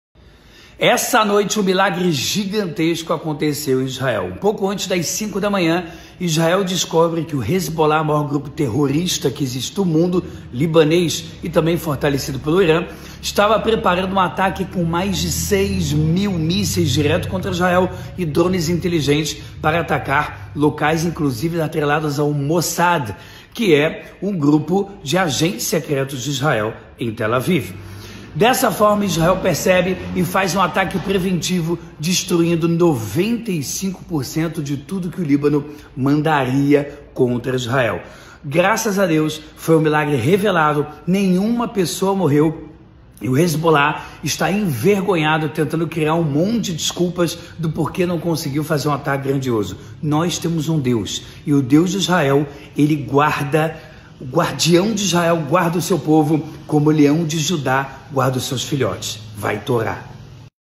6 mil mísseis contra Israel e faz um ataque preventivo destruindo 95% dos mísseis.
MILAGRE EM ISRAEL! Exército israelense descobre que o grupo terrorista Hezbolah iria enviar 6 mil mísseis contra Israel e faz um ataque preventivo destruindo 95% dos mísseis. O Deus de Israel não cochila nem dorme - salmo 121.